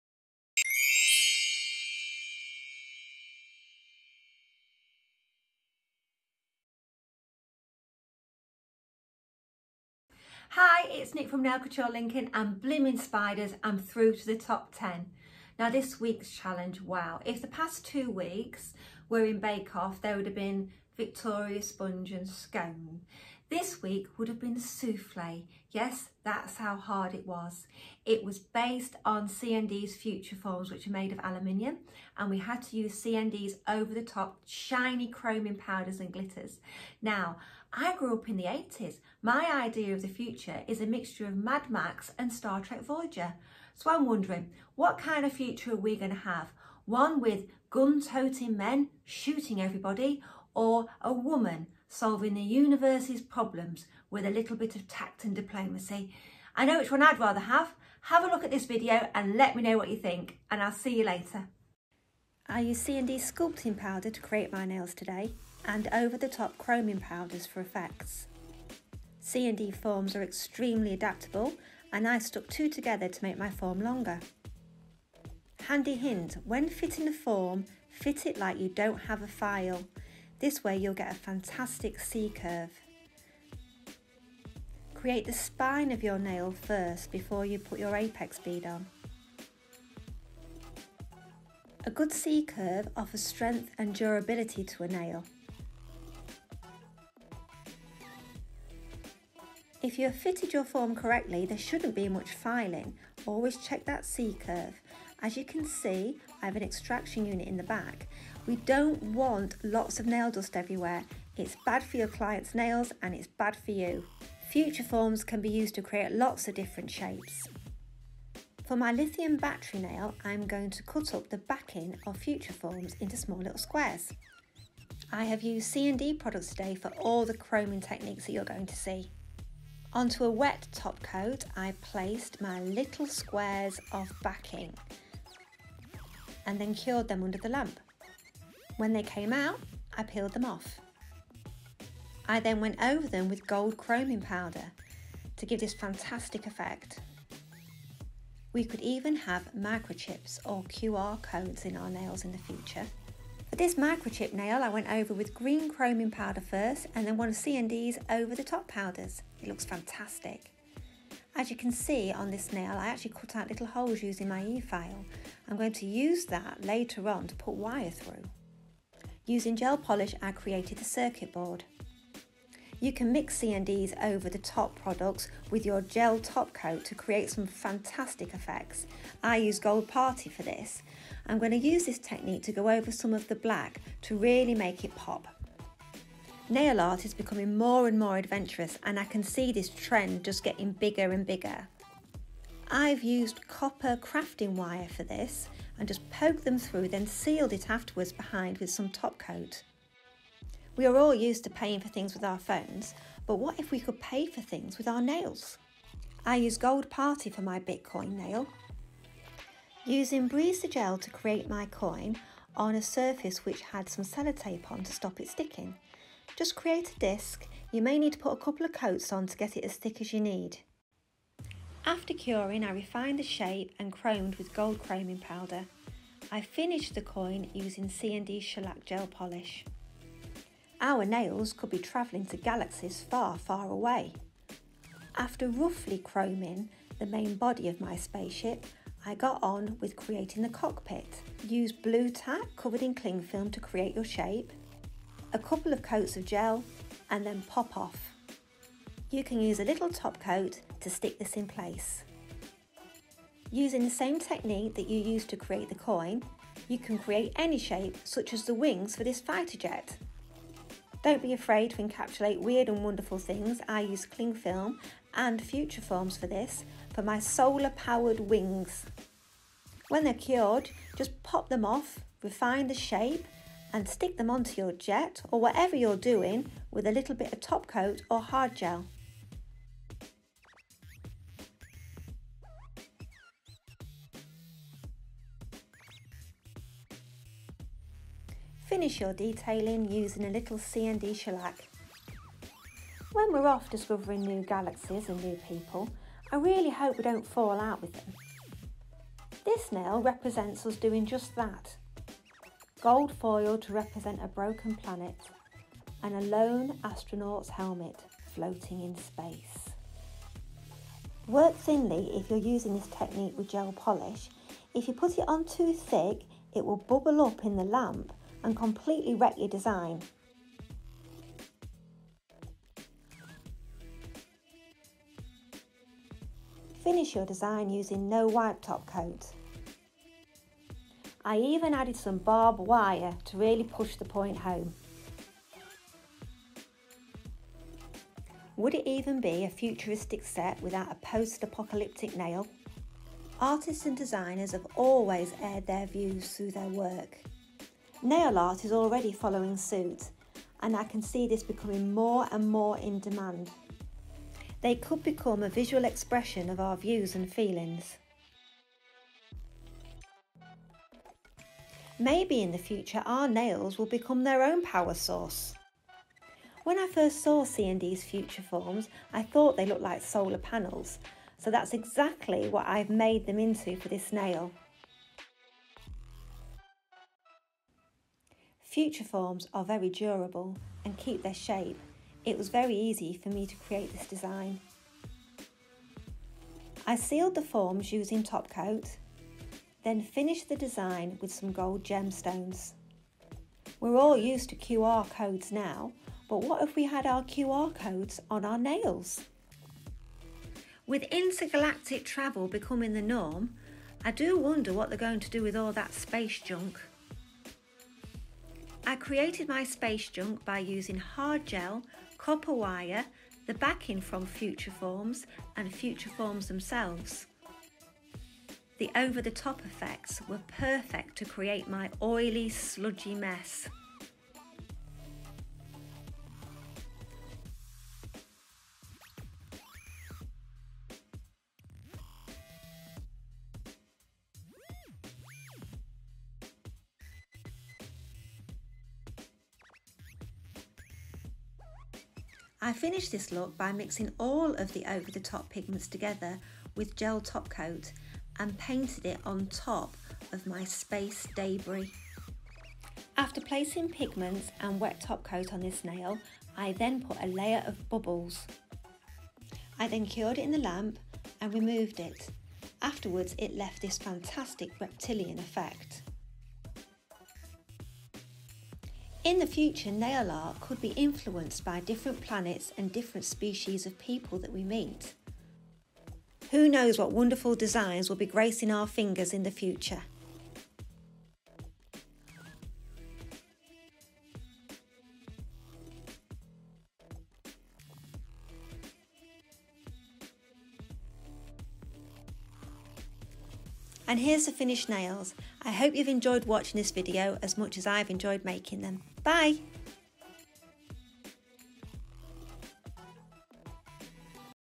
Challenge 3 NTNA2022 Over The Top Sponsored by CND
This week we were asked to create 10  futuristic nails using CND future forms . The brief included visible C-Curve , the use of CNDs Over The Top powders and only the use of metallic colours plus black and white ….. they took ages !